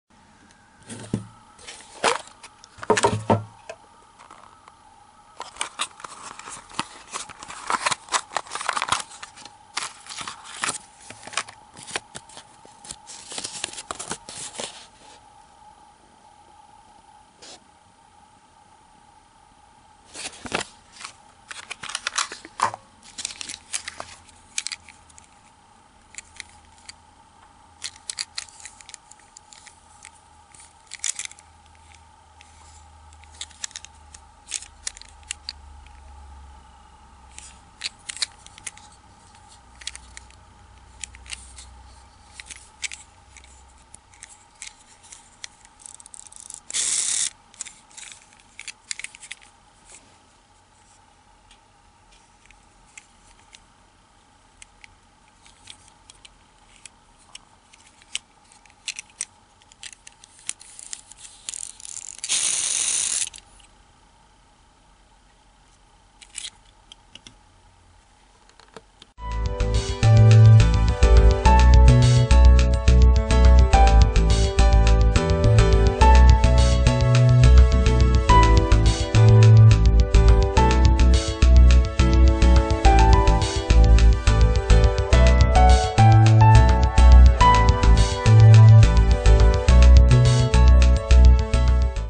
大型ハイキャリアー 2缶用 ミニミニチョロQコレクション FIRE 挽きたて微糖
FIRE 挽きたて微糖「ミニミニ チョロQコレクション」の2缶用 大型ハイキャリアーです。
キャンペーンは2013年4月2日（火）より。